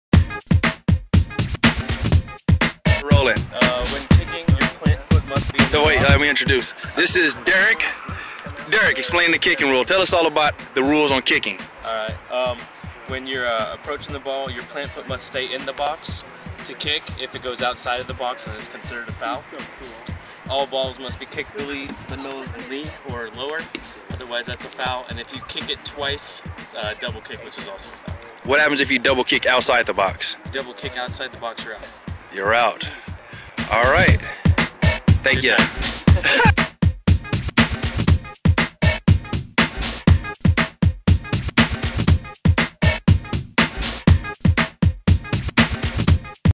WAKA Kickball AZ Thunder Rule Of The Week-Kicking
In this video Derek of the AZ Thunder Got Past 2nd Base explains the rule #9 "Kicking".

follow me @ProKickballer